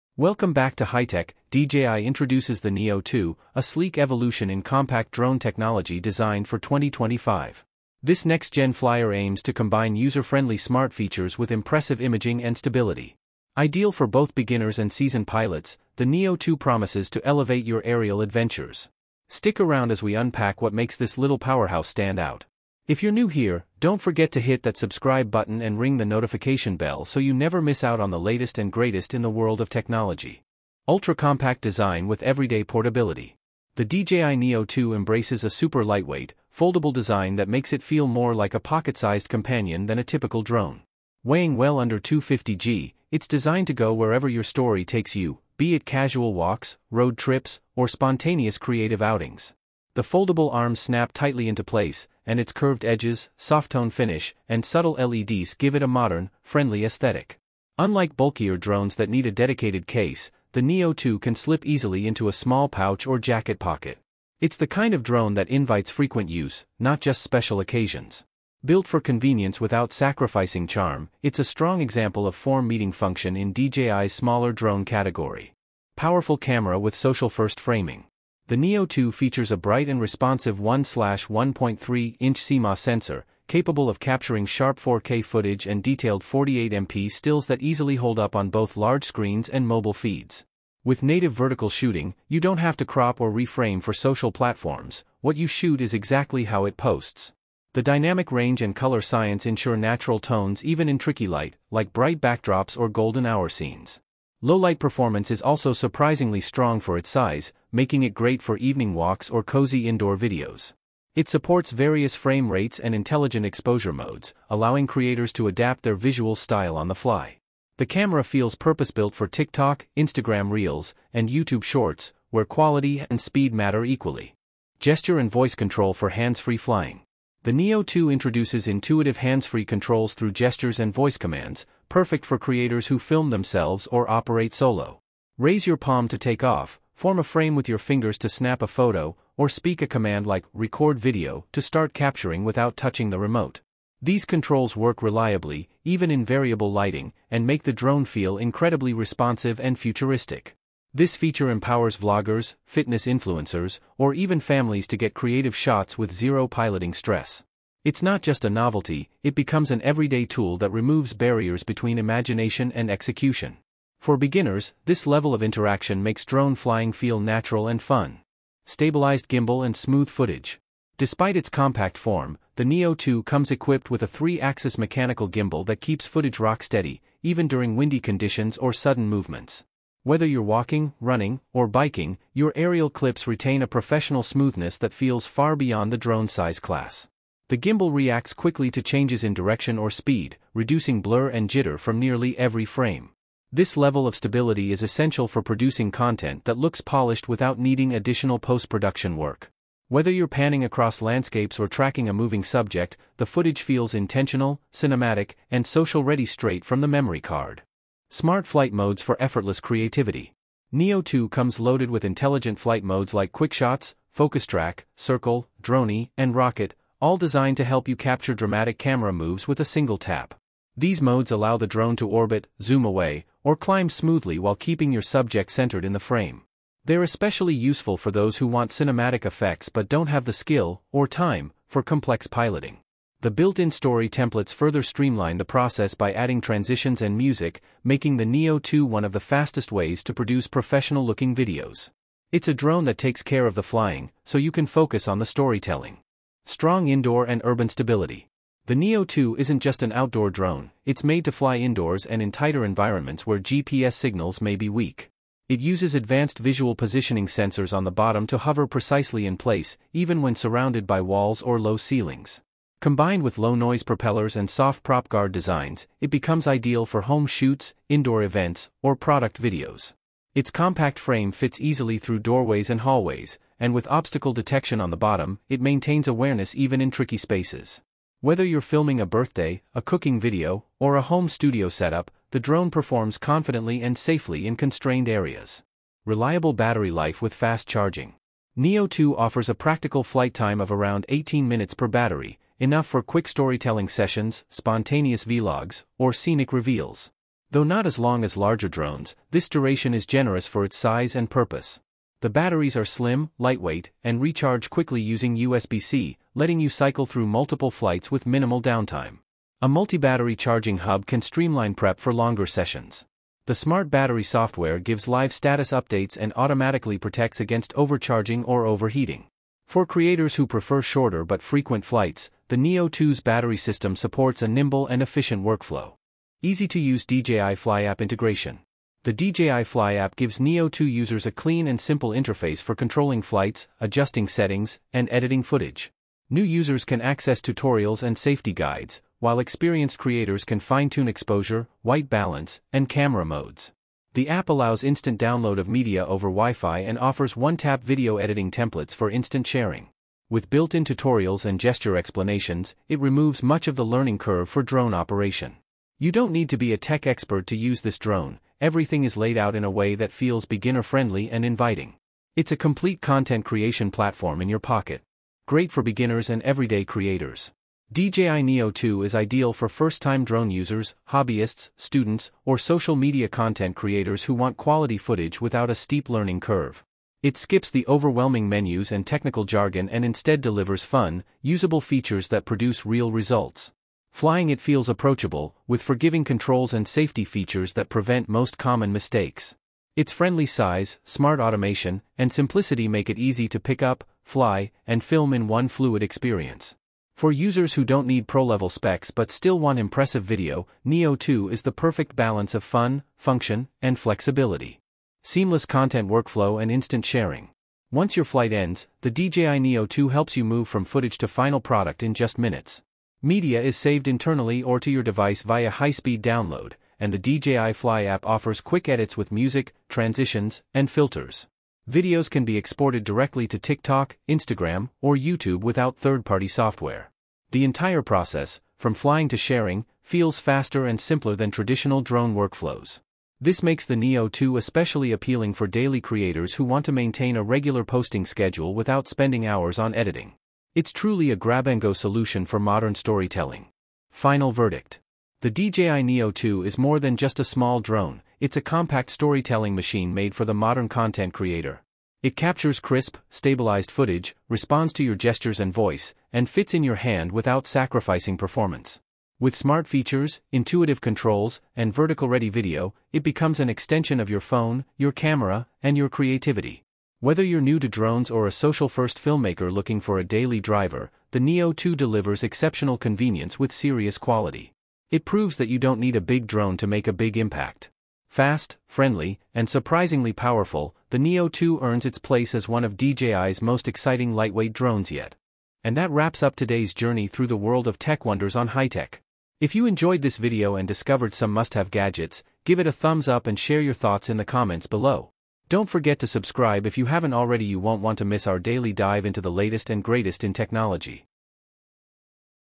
DJI Neo 2 Leaks - Full Specs, Price, and Release Date Revealed
DJI has officially revealed the Neo 2, and it’s shaping up to be one of the most impressive beginner-friendly drones of 2025. With a completely redesigned frame, the Neo 2 now includes a 1/1.3-inch sensor capable of shooting 4K at 60fps, smarter obstacle detection, and a longer 38-minute flight time. It's clear DJI wants this drone to be the new gold standard for casual flyers who still demand high-quality content.

Along with its upgraded camera and flight performance, the DJI Neo 2 introduces new automated flight modes and real-time subject tracking that rivals higher-end models. Priced competitively at around $699, the Neo 2 is set to launch globally in August 2025. Whether you're a first-time pilot or looking for a compact secondary drone, the Neo 2 is worth a serious look.